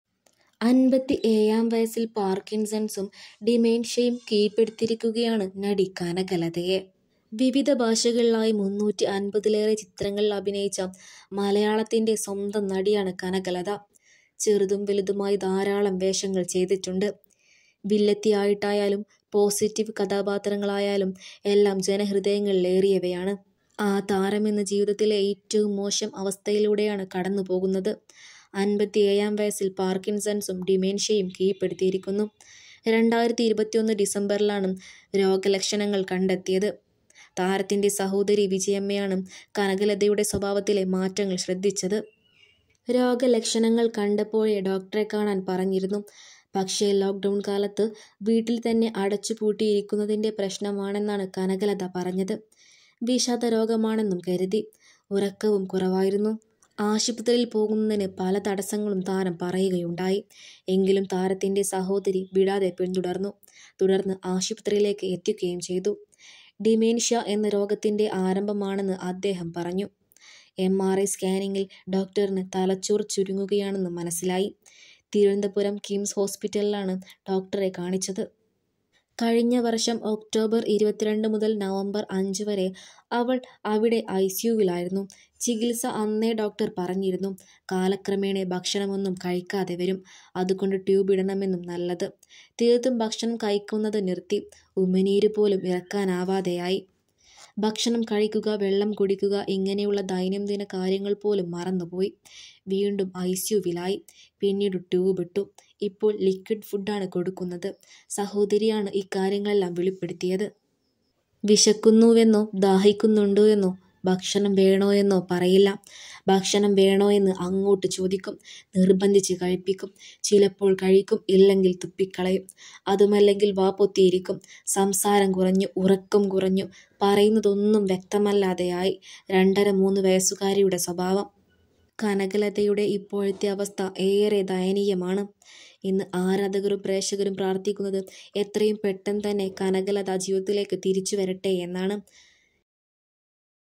ദൈവമേ ശത്രുക്കൾക്ക് പോലും ഇങ്ങനെ ഒരു അവസ്ഥ വരുത്തല്ലേ എന്ന് ആരാധകർ 🥲സ്വന്തം പേര് പോലും മറന്നു ഓർമ്മ
സ്വന്തം പേര് പോലും മറന്നു ഓർമ്മകൾ എല്ലാം നഷ്ടപ്പെട്ടു ട്രോപിലൂടെ ഭക്ഷണം നടി കനകലതയുടെ ഇന്നത്തെ അവസ്ഥ